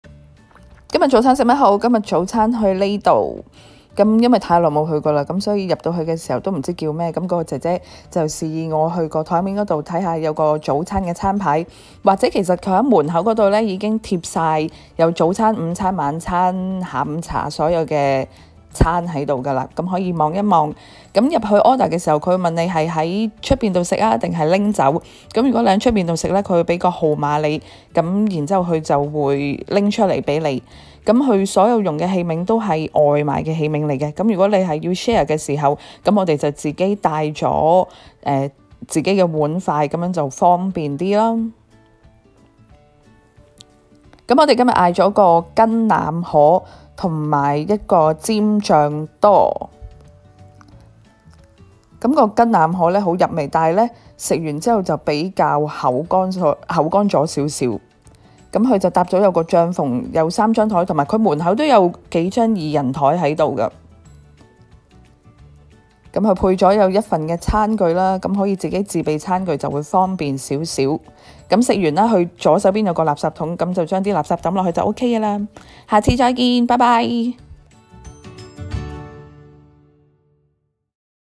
🇨🇦多倫多搵食記
占醬多
牛筋腩河
跟餐茶x2

藍海餐廳 Blue Ocean Cafe
地址：8392 Kennedy Rd a6, Unionville, Ontario L3R 0W4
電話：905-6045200